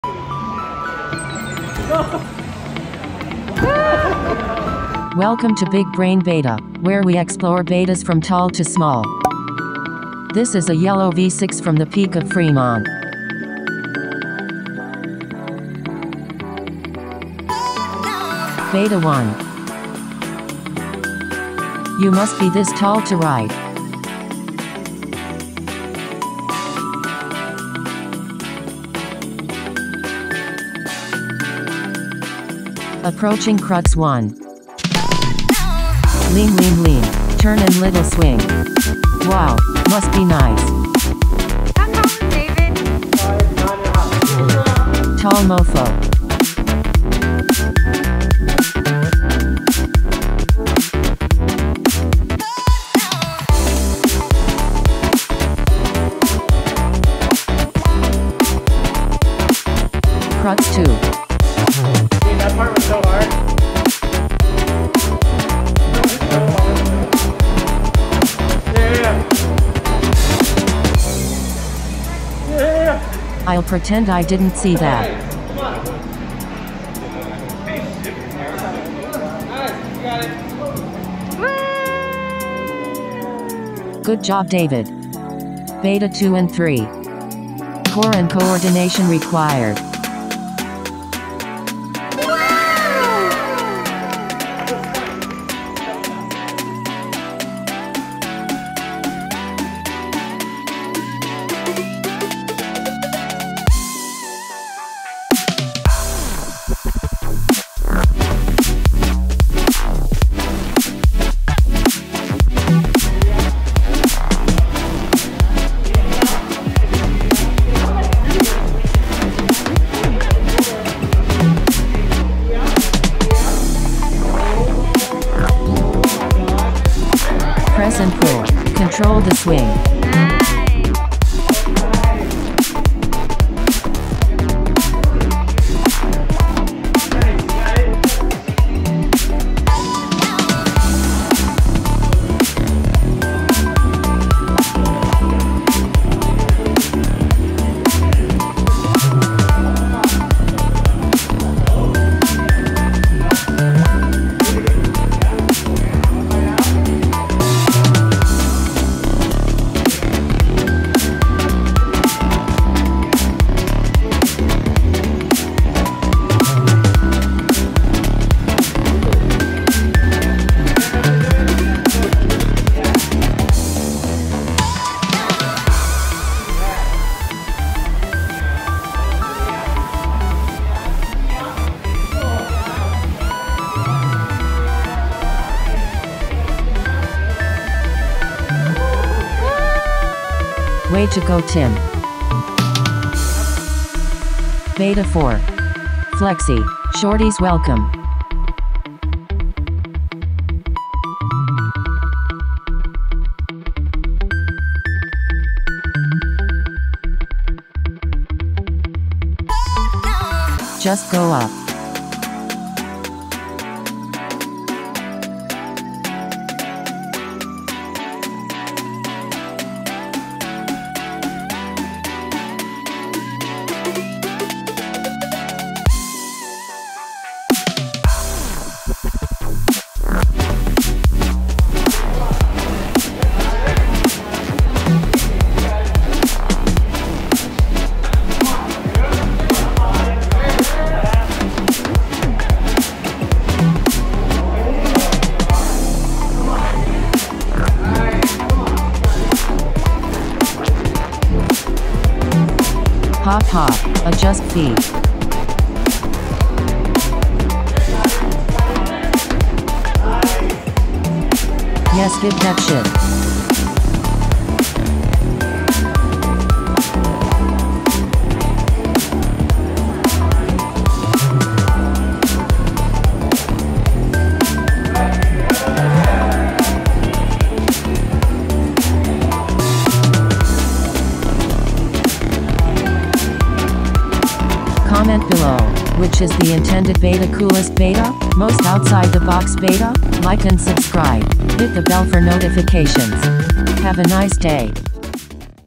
Different beta for different bodies- Yellow V6 | Big Brain Beta
One V6, three climbers, four betas!

0:00 Intro
0:19 David (5'10")
1:25 Tim (5'5")
3:14 Tofu (5'1")

Special thanks to David and Tim for their beta for this video. Thanks Jerry for the spot!
And special thanks to my bud, Lety, for giving me the Big Brain Beta (BBB) name!